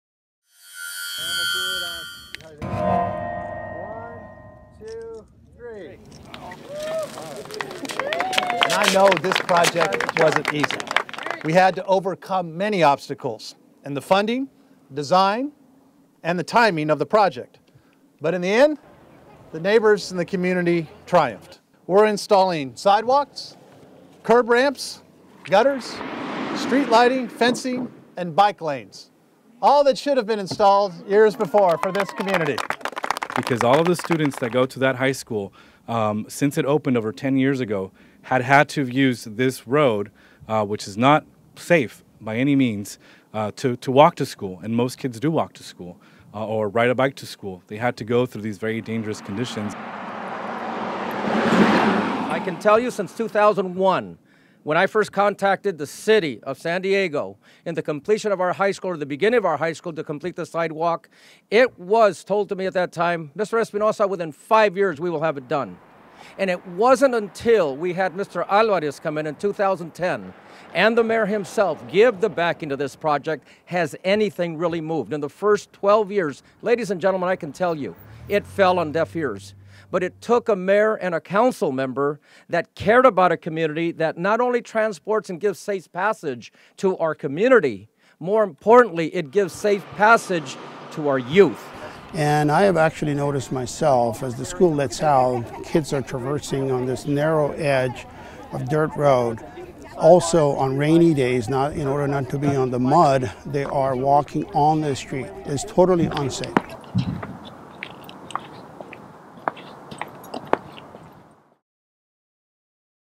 Building Safe Sidewalks in San Ysidro
Crews will begin work on Old Otay Mesa Road to add bike lanes, guard rails, sidewalks and street lights, making it more safe for students walking to nearby San Ysidro High School.